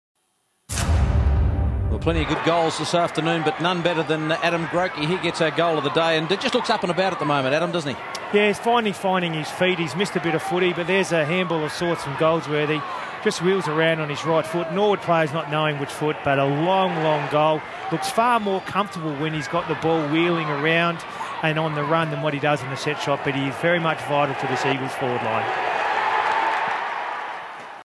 SANFL 2011 Preliminary Final Goal of the Day - Adam Grocke
SANFL 2011 Preliminary Final Goal of the Day - Adam Grocke - Eagles. Norwood v Eagles. This match was played at Football Park on Sunday Oct 2nd 2011. The winner meets Central District in the Grand Final on October 9th 2011. Commentary from Michael Maney and Andrew Rogers.